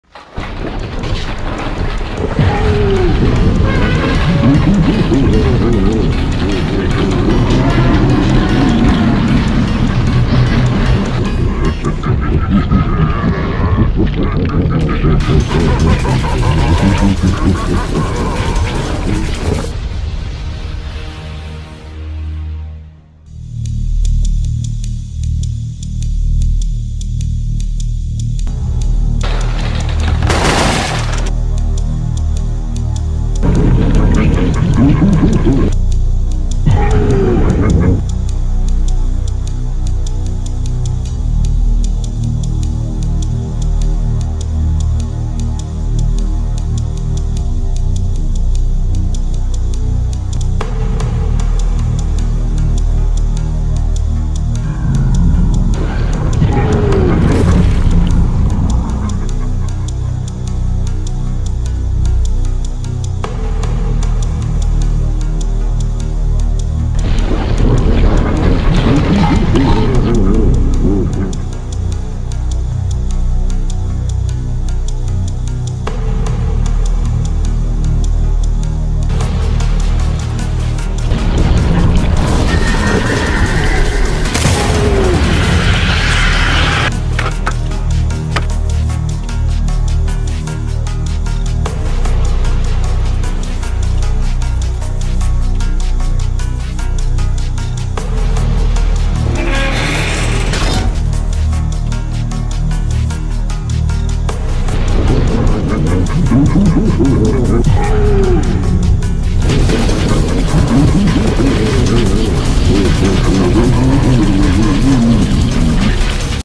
Doors: Average grumble encounter [Moon animator] Roblox animation (Credits in description)
Been roughly 120+ days lol

Side characters:
Tayorluca: @Taylorluca
Sniffington: @sniffingtonn

9.5k keyframes… FOR THIS?!